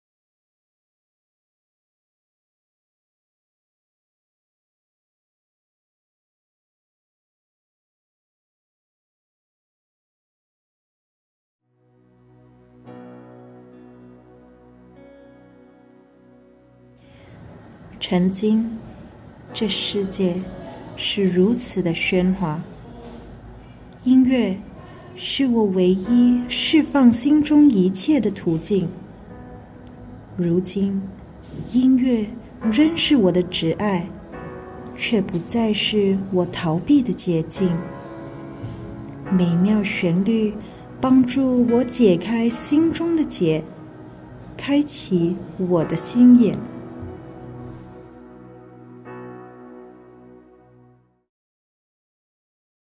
The Sacrifice Teaser #1
Greetings of Easter 2020 from Hope Serdang Church. This year, we are convicted to bring to you, a real-life testimony, which outlines the sacrifice of our Lord Jesus Christ that brought freedom, joy, peace and salvation.

Due to the current MCO, we will not be able to gather for Easter. But fret not, we will release this short film testimony online.

Date: 11 April 2020
Time: 8pm
Venue: Hope Serdang Church FB Live